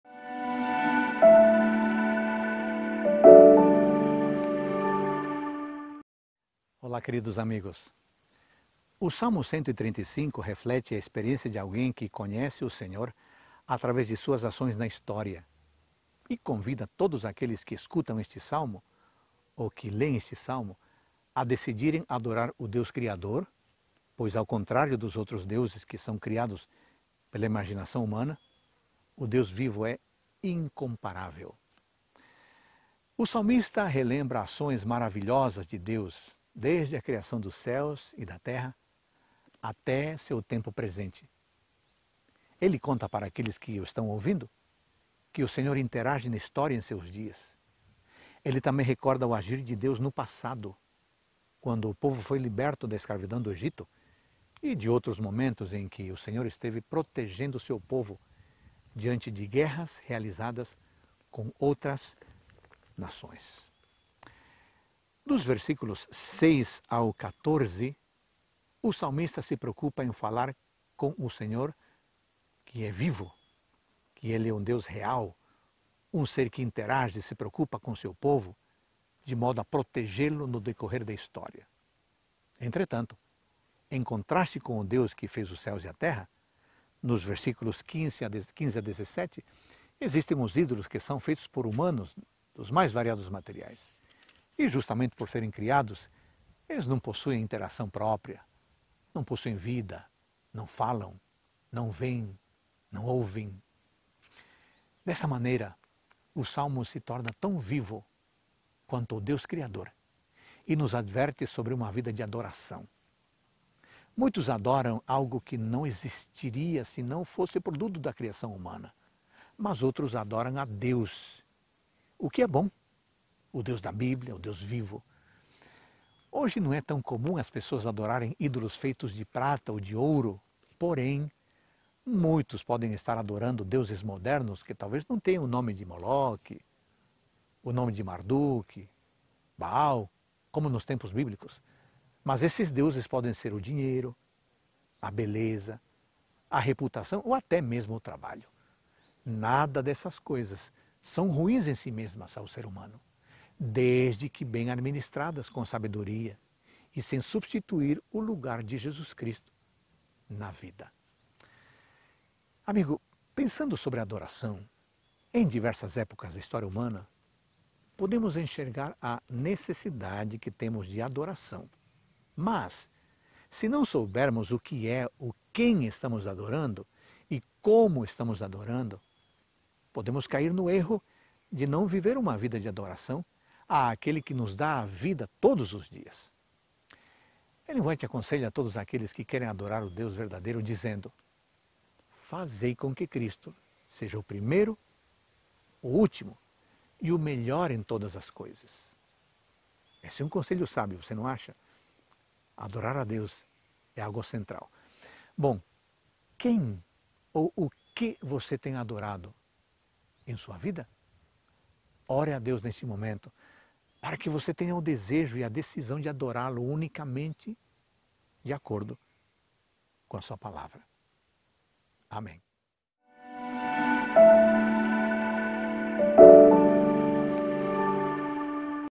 Salmos 135 - Reavivados Por Sua Palavra
O Salmo 135 reflete a experiência de alguém que conhece o Senhor através da sua ação na história.